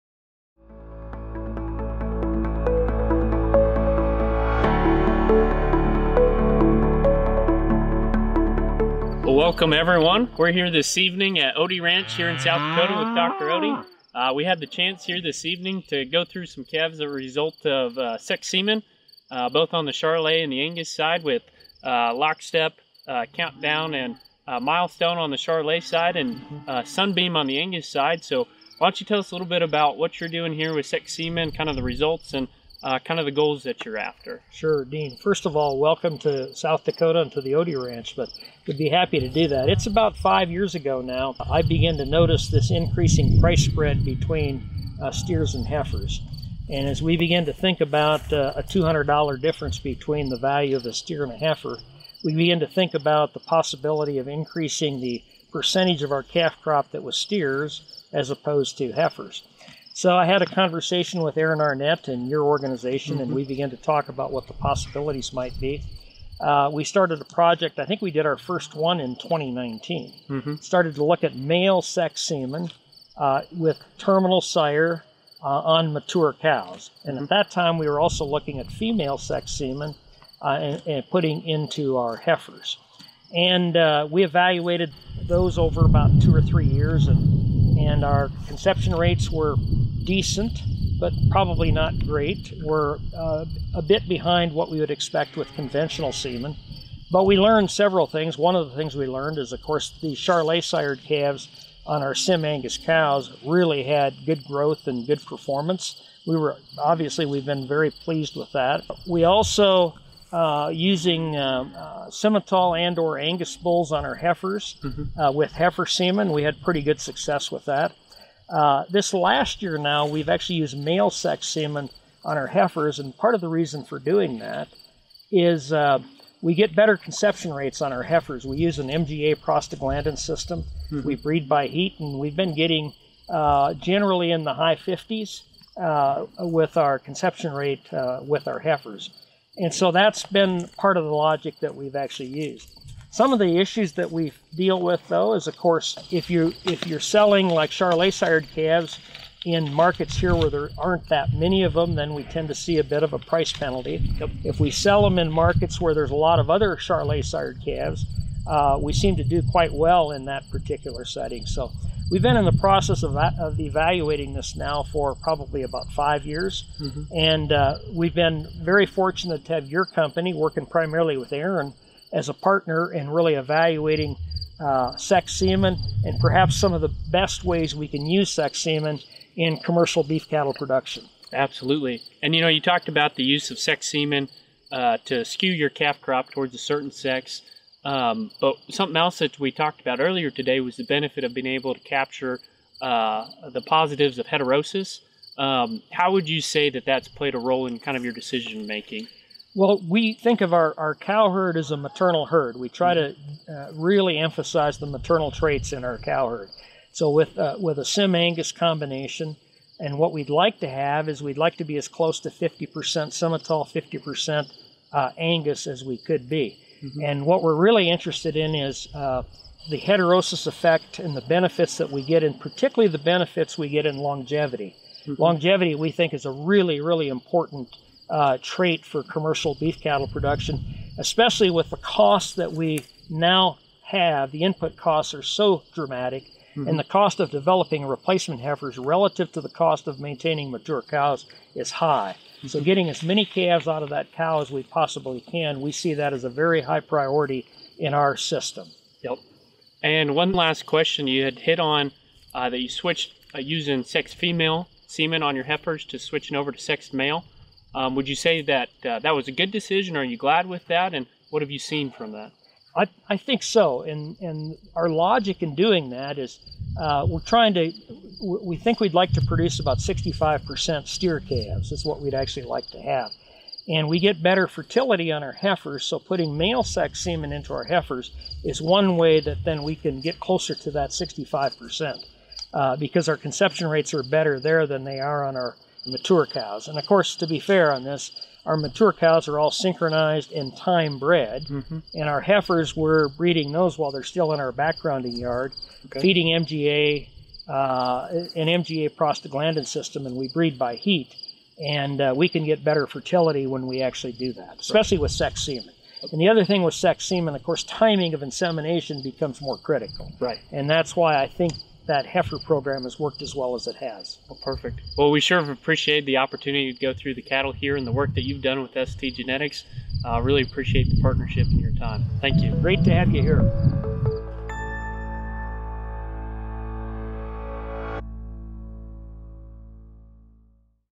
Odde Ranch Interview
STgenetics® Beef Regional Manager, Dean Klahr, visited Odde Ranch in South Dakota! Dean and Dr. Ken Odde, Professor Emeritus at Kansas State University and Owner of Odde Ranch, discuss strategic use of sex-sorted genetics. Dr. Odde talks about his projects with STgenetics® Beef, and how our partnership in evaluating sexed semen has resulted in finding some of the best ways we can utilize male and female sex-sorted semen in commercial beef cattle production.